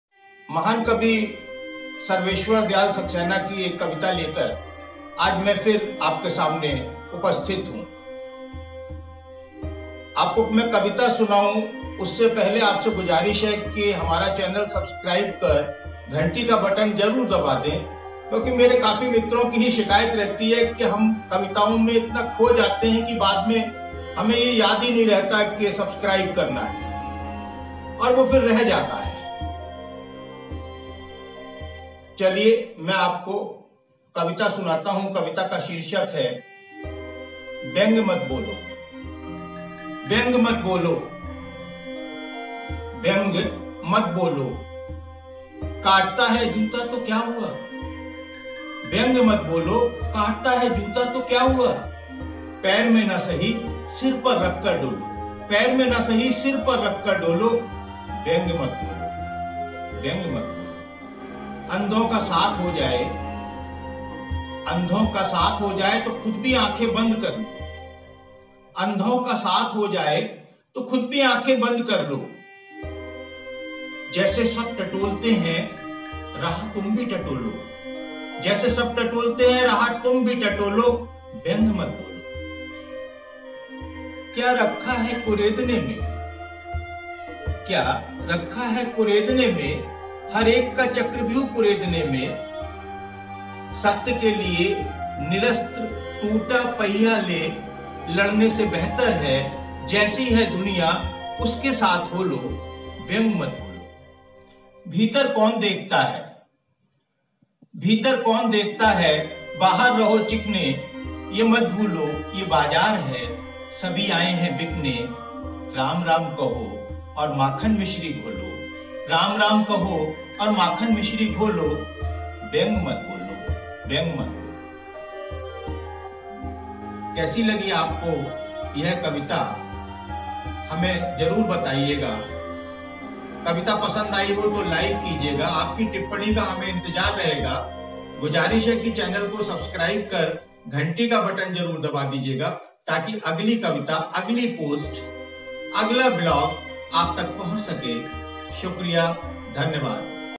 best poem ever in hindi by sarveshwar Dayal Saxena, व्यंग्य मत बोलो
आधुनिक हिंदी कविता के सशक्त हस्ताक्षर सर्वेश्वर दयाल सक्सेना की व्यंग्य कविता ‘व्यंग्य मत बोलो’ इर्द-गिर्द में प्रस्तुत है।
Vyangya Mat Bolo is one of the best poem ever in hindi. Sarveshwar Dayal Saxena is well known author of modern literature in Hindi language. Hope you will like to listen this poem on common people in Hindi.